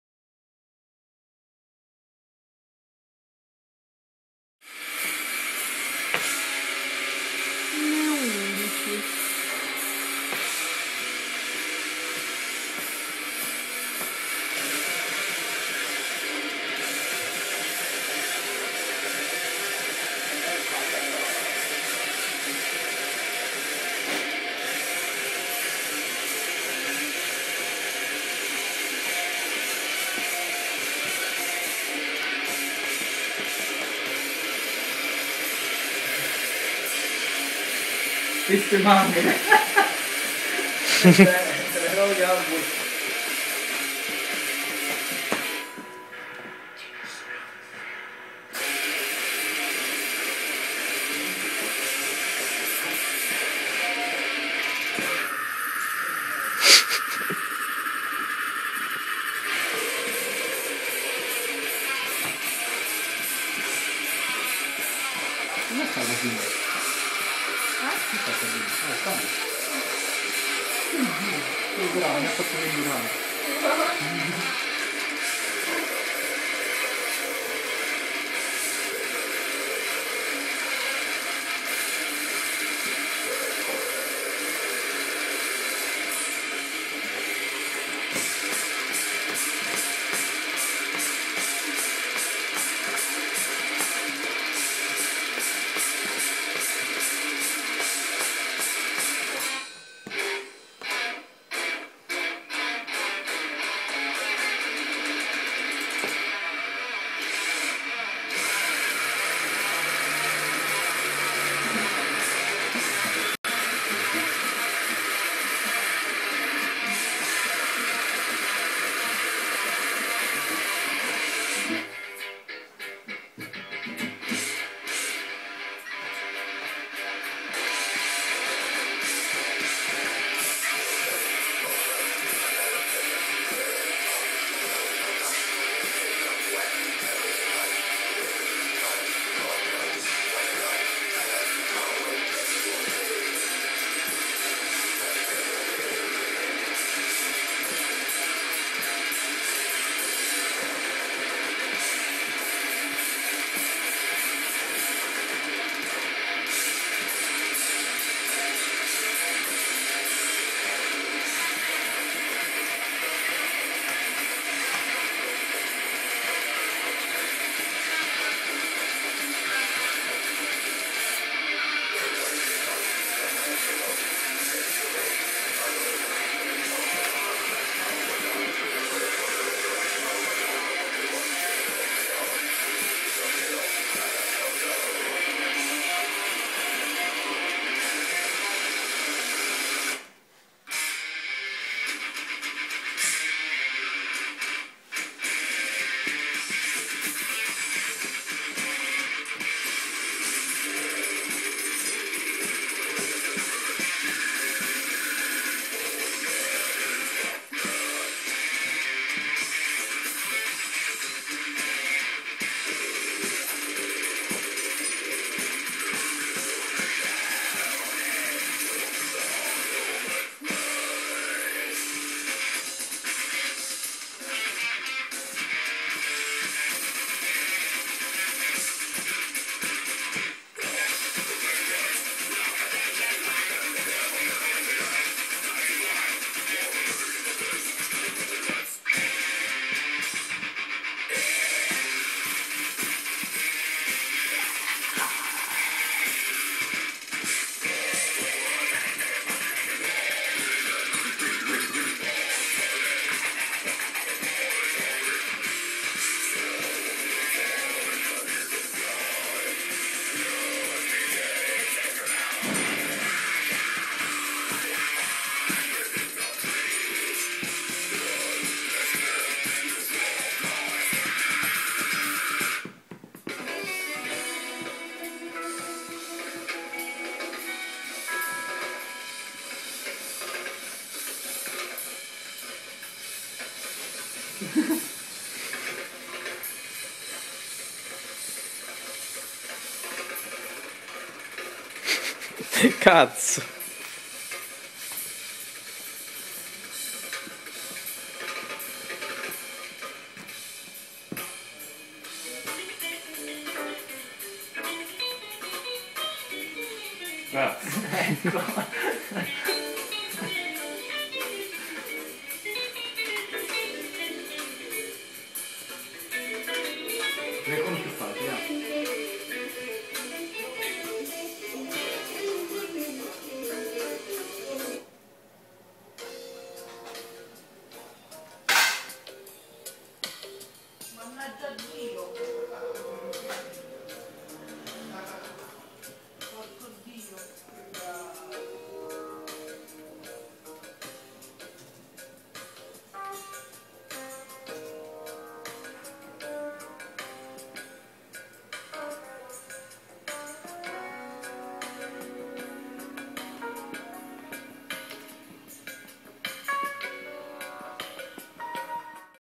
Attila air drummer
il grandissimo attila in una delle sue migliori performance improvvisate